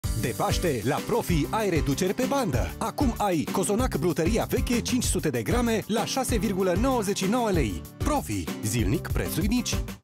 Acum ai Cozonac Brutăria Veche 500g la un pret special.
De Paste la Profi ai reduceri pe banda!! Acum ai Cozonac Brutăria Veche 500g la un pret special. Oferta supusa unor termene si conditii. Profi, zilnic preturi mici.